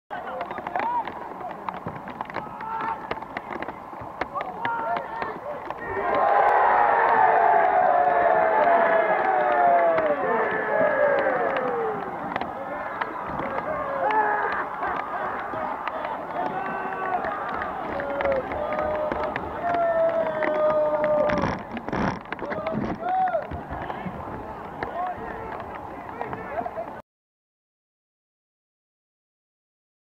AFC Hornchurch 1 Tilbury 1 (31 Jan 17) - Conway goal
Reece Conway equalises for Tilbury